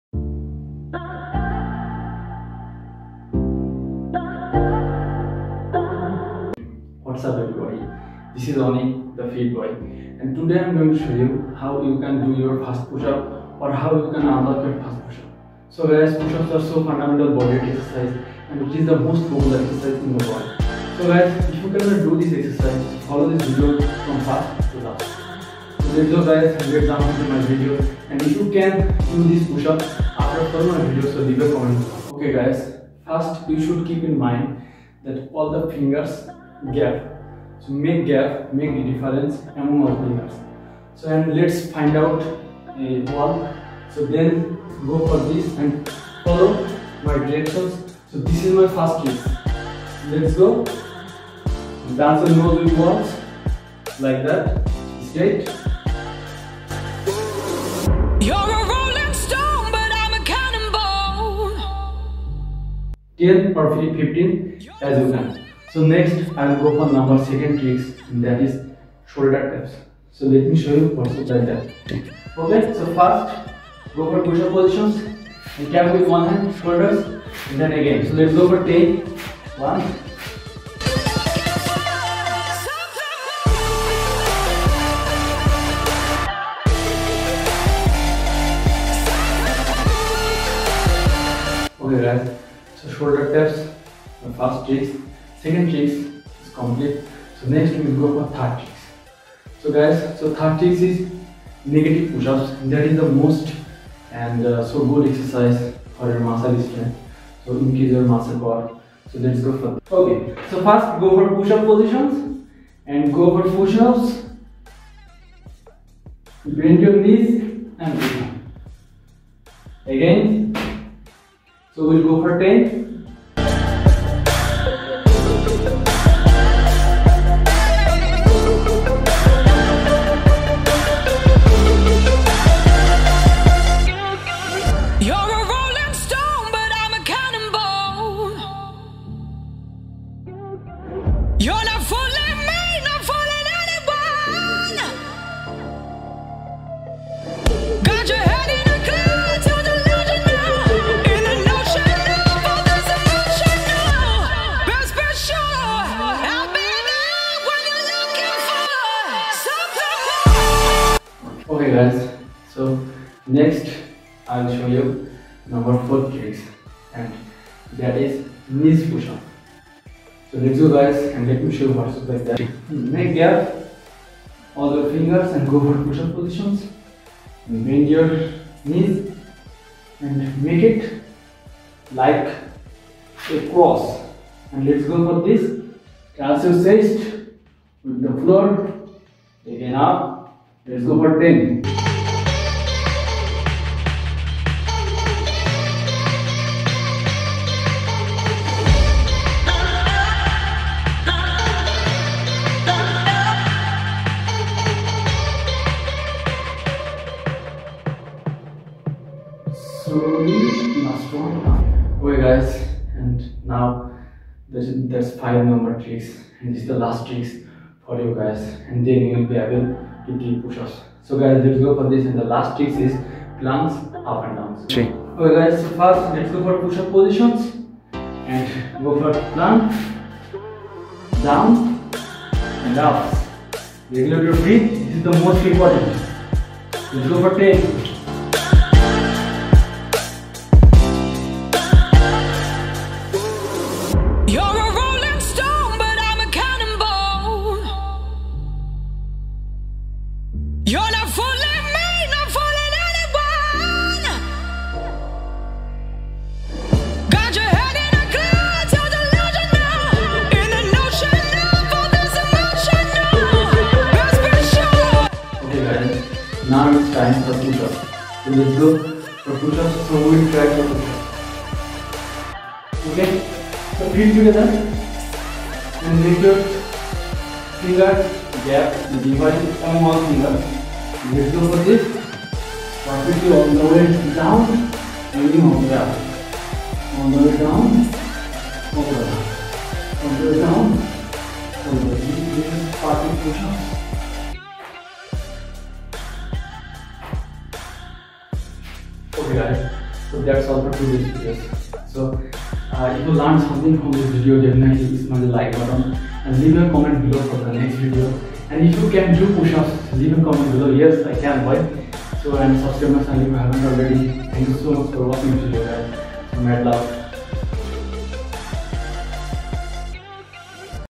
Unlock Your First Push Up (If You Can't Do Push Up).
Anik The Fit Boy Shows You How You Can Unlock Your First Push -Up . If You Can't Do Push -up .
Add More:
UnlockYourFirstPush-up
HowToLearnPush-Up